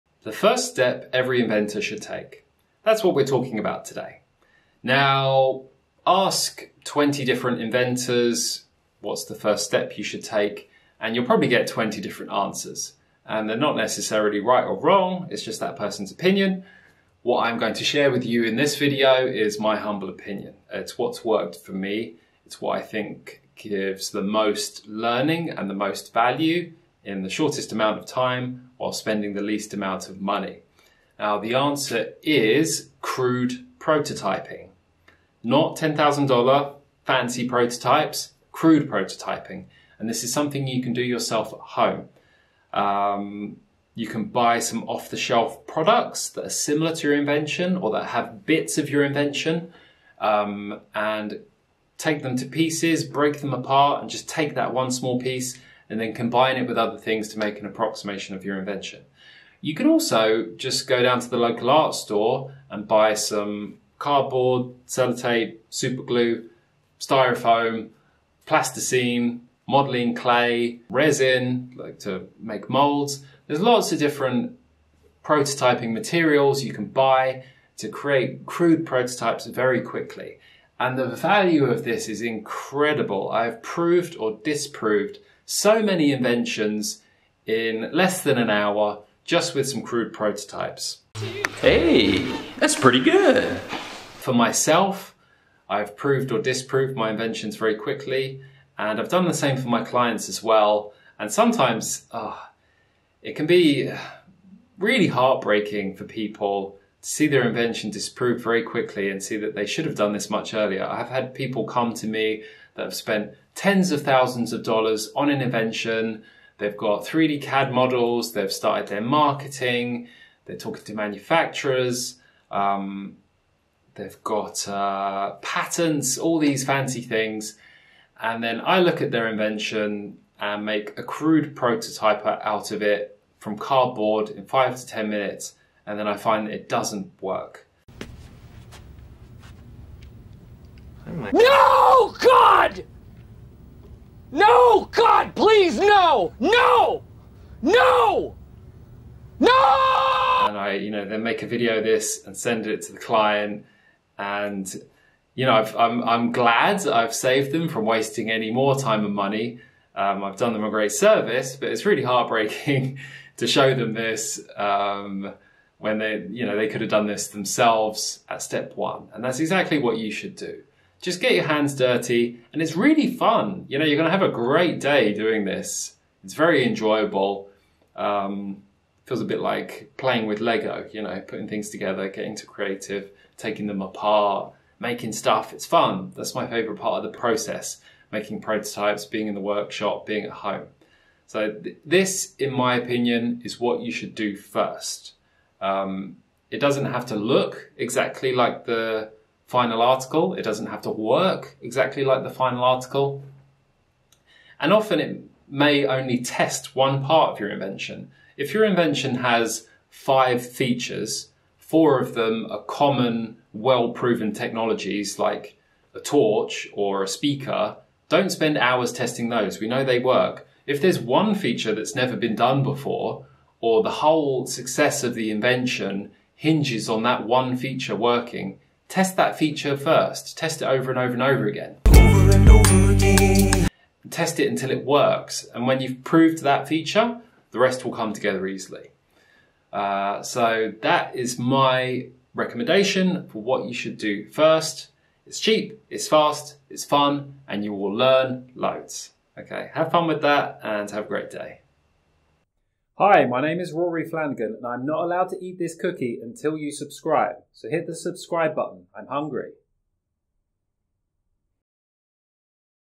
The First Step Every Inventor Should Take
The First Step Every Inventor Should Take is crude prototyping.
You should take this critical step before investing time, energy, and money into your ideas. In this video, learn several different methods for creating prototypes for very little money. By taking this initial step, you'll be able to test the functionality of your invention and make any necessary adjustments before moving forward. Don't miss this essential guide for inventors of all levels!

⏱️⏱️VIDEO CHAPTERS⏱️⏱️
0:00 - The First Step Every Inventor Should Take
0:30 - Crude Prototyping
1:24 - Prove or disprove your invention in less than an hour
3:25 - Test the most critical part of your invention
4:00 - Summary

✅ For free guides on how to make sure your idea is protected the right way, check out our inventor resource library ➡️